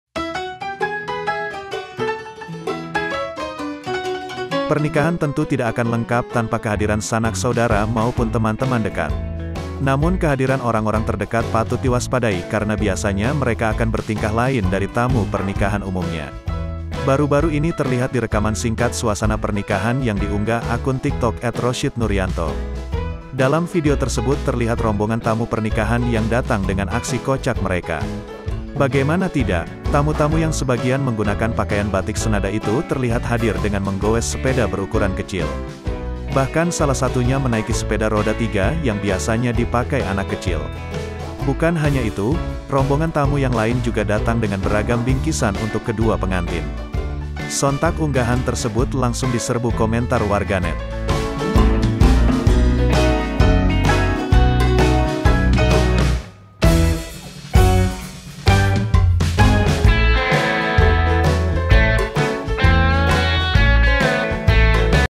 Viral Aksi Kocak Rombongan Tamu Pernikahan Naik Sepeda Roda Tiga
aksi kocak | tamu undangan | naik sepeda roda tiga | Berita Nasional | Video Viral
TRIBUNLAMPUNG - Viral Aksi Kocak Rombongan Tamu Pernikahan Gunakan Sepeda Roda Tiga
Pernikahan tentu tidak akan lengkap tanpa kehadiran sanak saudara maupun teman-teman dekat.
Namun kehadiran orang-orang terdekat patut diwaspadai karena biasanya mereka akan bertingkah lain dari tamu pernikahan umumnya.
Baru-baru ini terlihat di rekaman singkat suasana pernikahan yang diunggah akun TikTok @rosyidnuryanto.
Dalam video tersebut terlihat rombongan tamu pernikahan yang datang dengan aksi kocak mereka.
Bagaimana tidak? Tamu-tamu yang sebagian menggunakan pakaian batik senada itu terlihat hadir dengan menggowes sepeda berukuran kecil.
Bahkan salah satunya menaiki sepeda roda tiga yang biasanya dipakai anak kecil.
Bukan hanya itu, rombongan tamu yang lain juga datang dengan beragam bingkisan untuk kedua pengantin.
Sontak unggahan tersebut langsung diserbu komentar warganet.